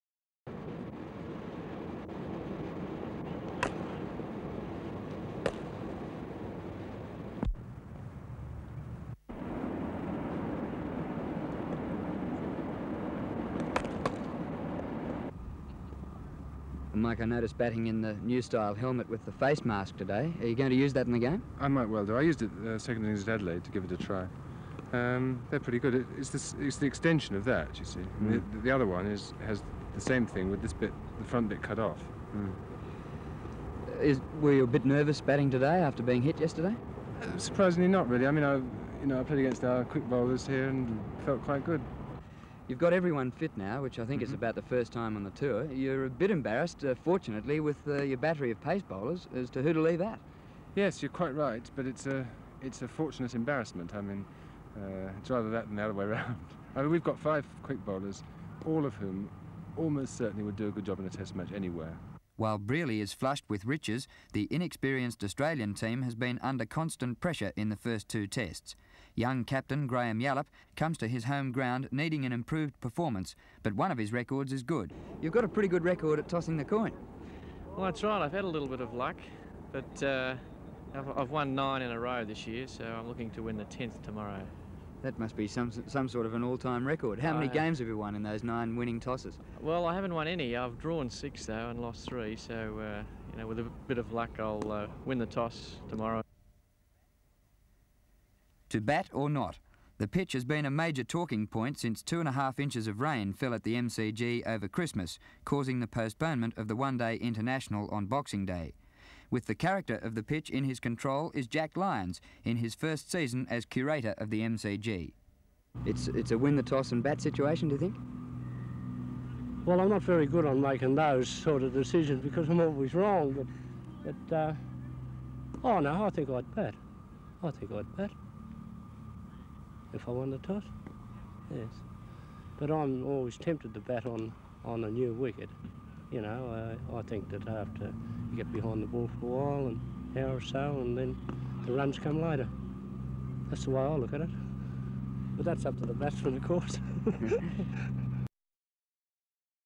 Australia V England 1978-79 Melbourne Test Preview
Interviews with Australian and English players in 1978 at the Melbourne Cricket Ground